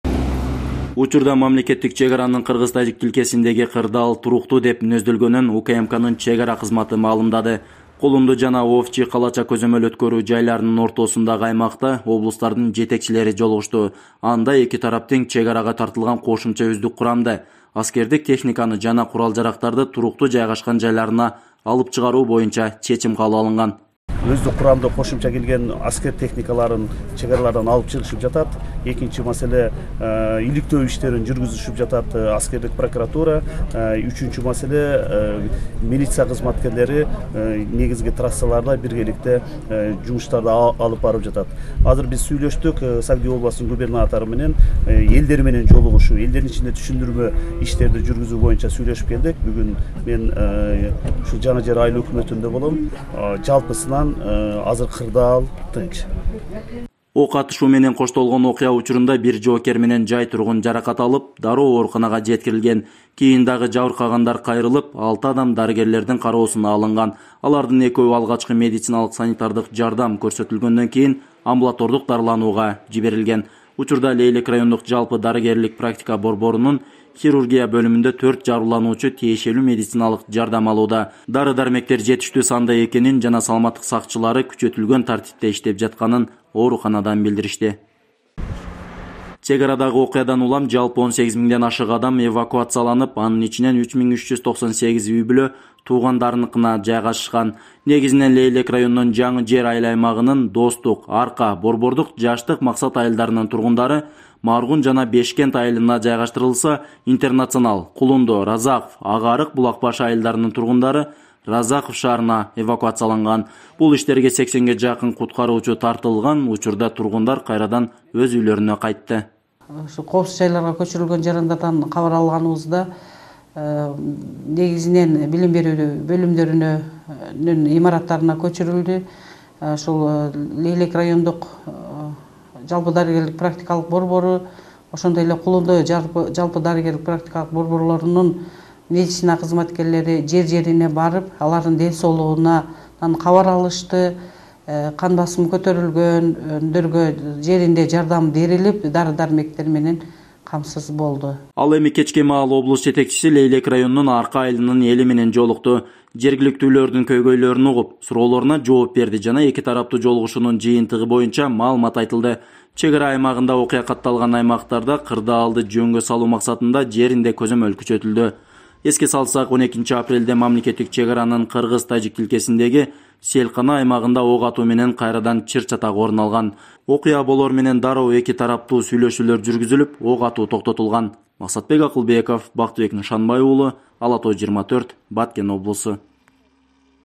Лейлек жана Баткен райондорунда абал туруктуу жана тийиштүү күч түзүмдөрүнүн көзөмөлүндө
Түндөгү орун алган кыргыз-тажик чек арасындагы окуядан кийин Лейлек жана Баткен райондорунда абал туруктуу жана тийиштүү күч түзүмдөрүнүн көзөмөлүндө. Ал эми коопсуз жайларга эвакуацияланган айыл тургундары өз үйлөрүнө кайтышты. Окуя болгон жерде эки өлкөнүн облустарынын жетекчилери жолугушуп, сүйлөшүүлөр жүрдү. Анын жыйынтыгы менен чек ара тилкесиндеги абалды туруктуу сактоо боюнча макулдашууларга жетишилди. Учурда Чек ара кызматы кыргыз-тажик тилкесин көзөмөлдөп турат.